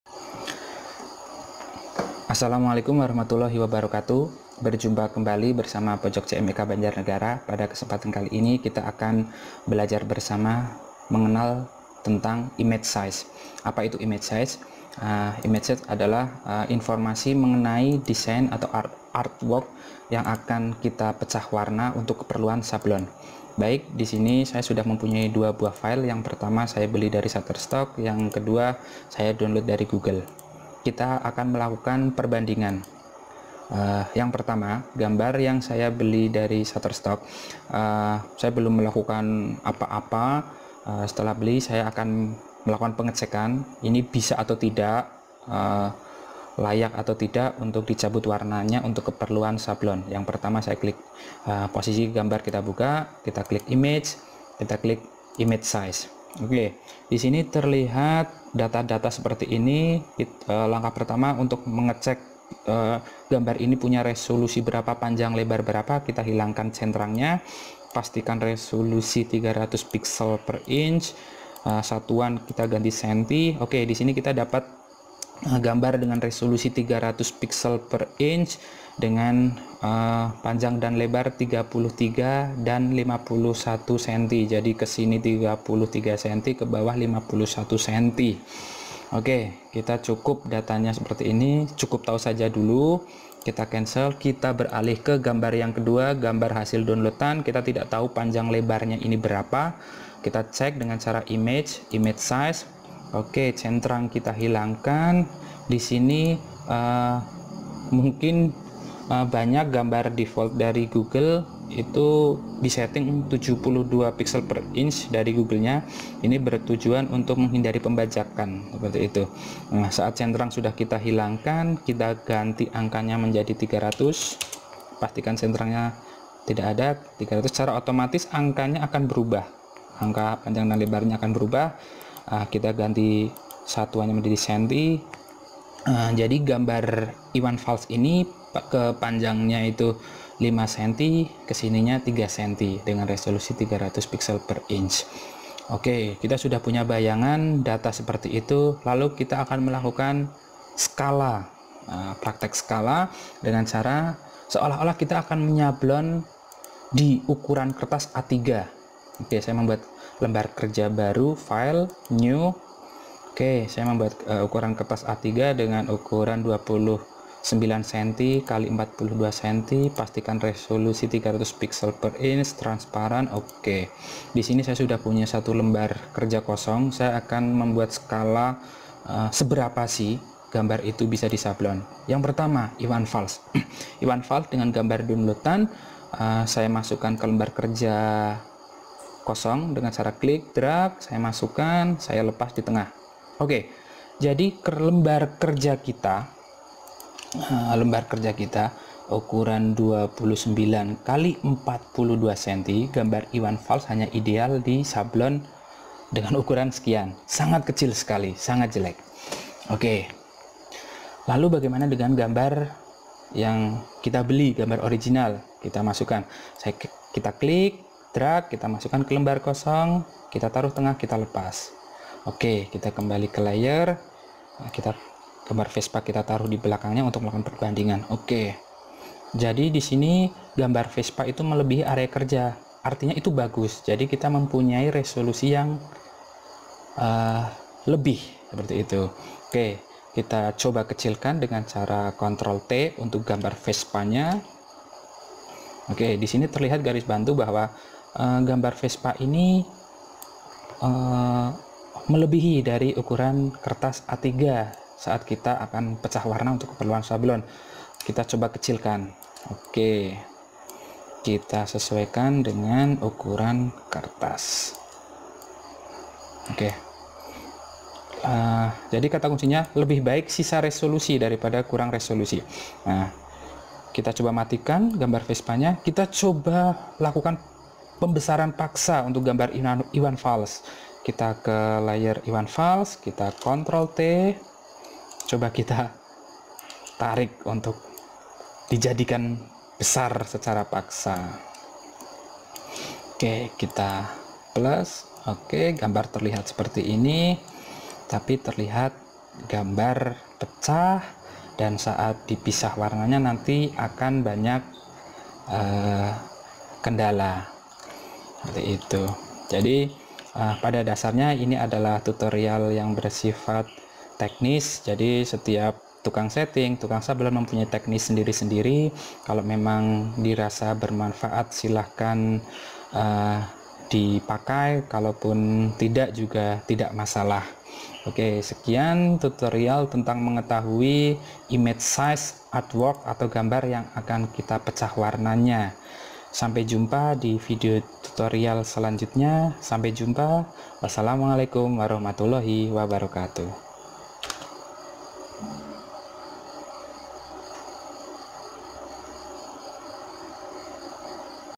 mengenal dan memahami "image size" dalam proses sablon separasi - (bahasa indonesia)
sedikit penjelasan tentang image size pada photoshop sebagai salah satu tahapan penting dalam proses sablon separasi.

video dibuat berdasarkan opini pribadi dan sumber-sumber yang menurut admin relevan :D .